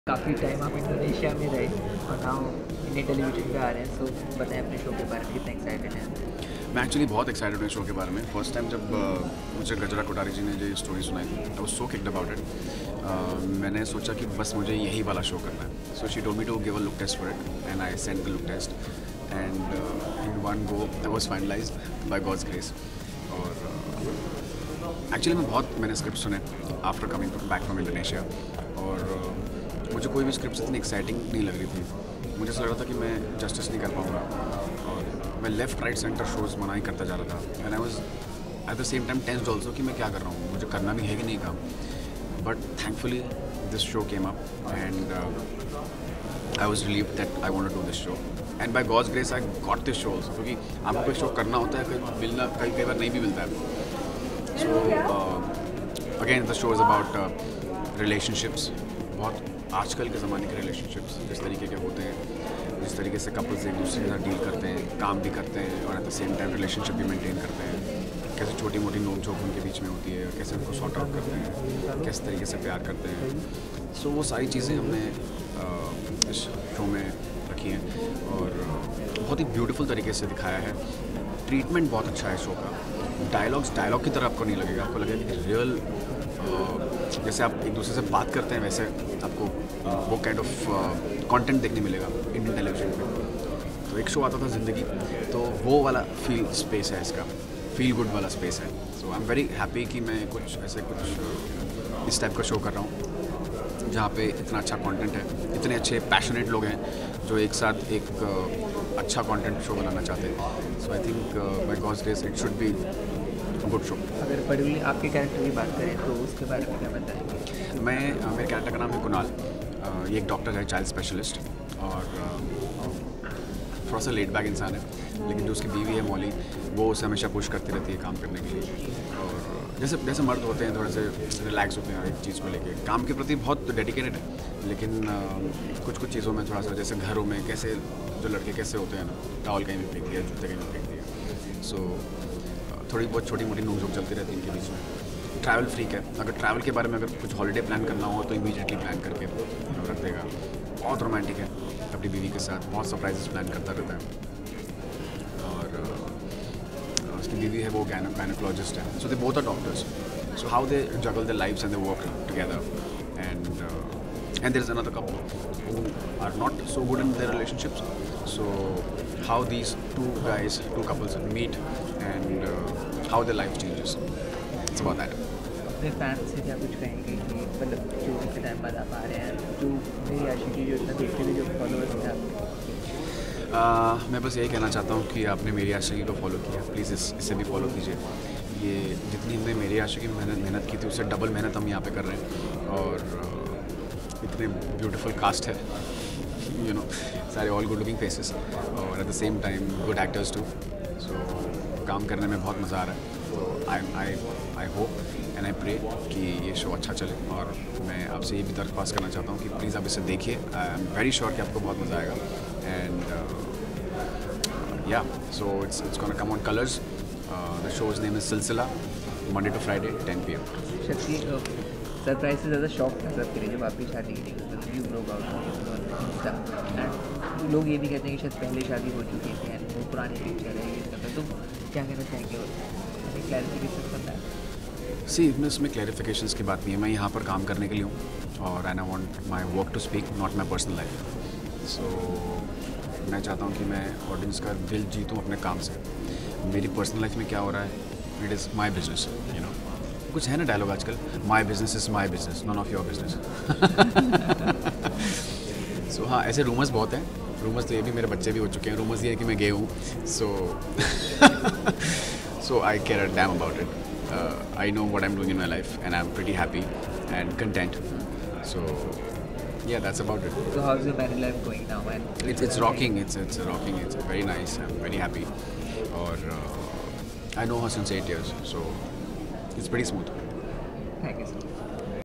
Shakti Arora Shares His Excitement About Silsila Badalte Riston Ka, Message For MATSH Fans & More
Shakti Arora's Storng reply when asked about his Marriage Photos being Old. Silsila Badalte Riston Ka on Colors TV, message for Meri Aashiqui Ab Tum Se Hi fans & how he got the show